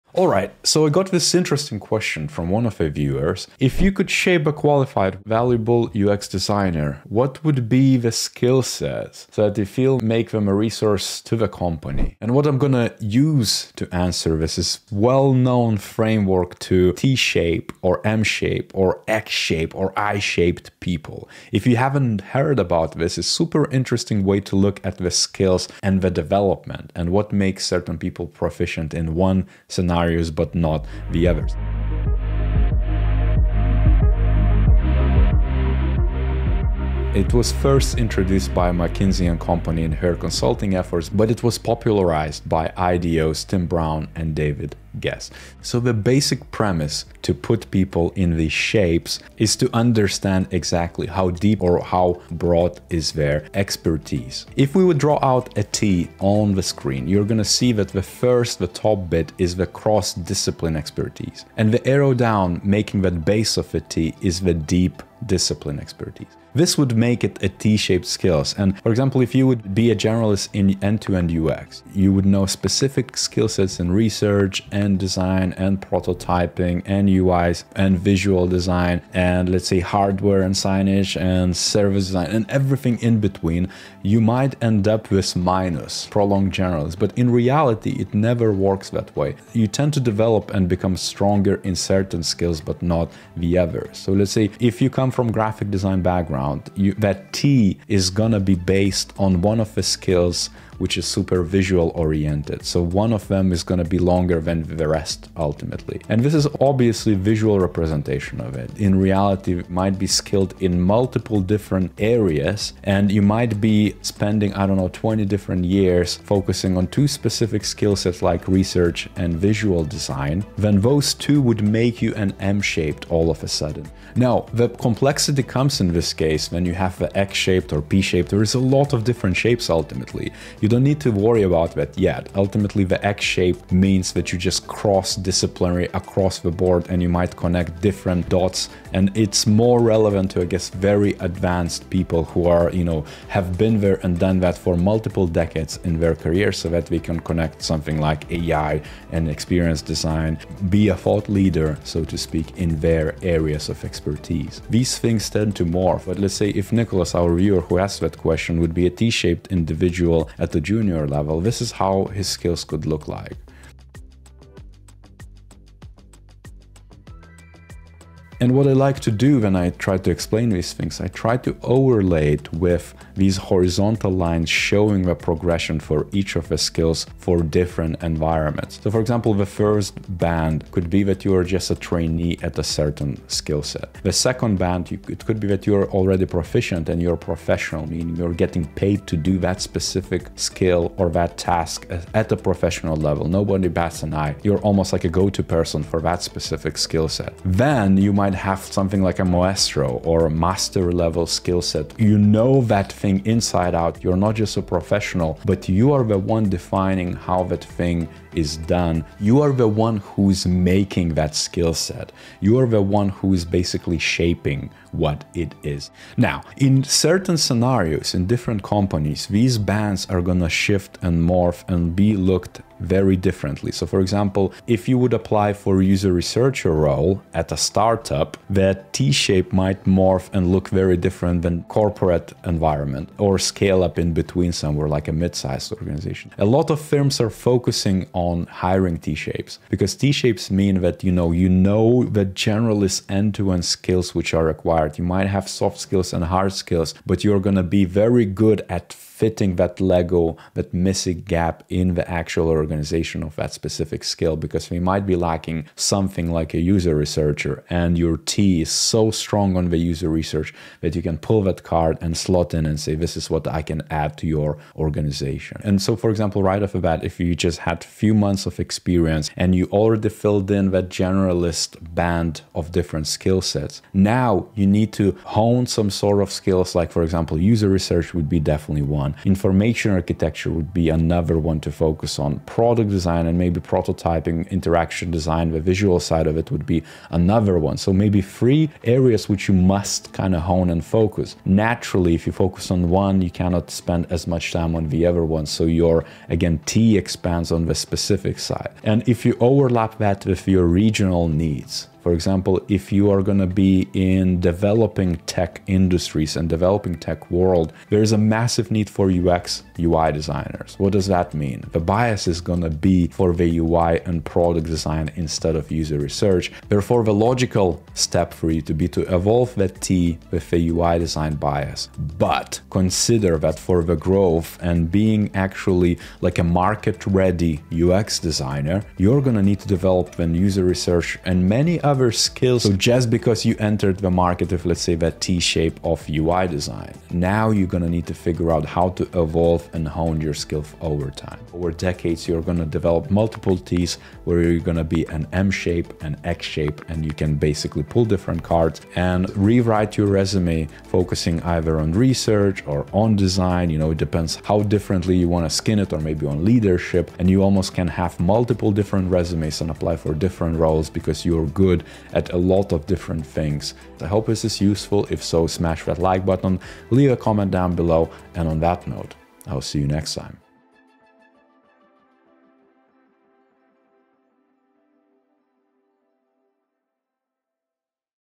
How to Develop Your UX Skills to be Hireable and Effective (the I, M, X... and T Shaped Designers)
In this video, I'll answer a recent question from one of the viewers on developing and shaping their UX design skills: what do companies look for in candidates and how one can optimise their development to be effective.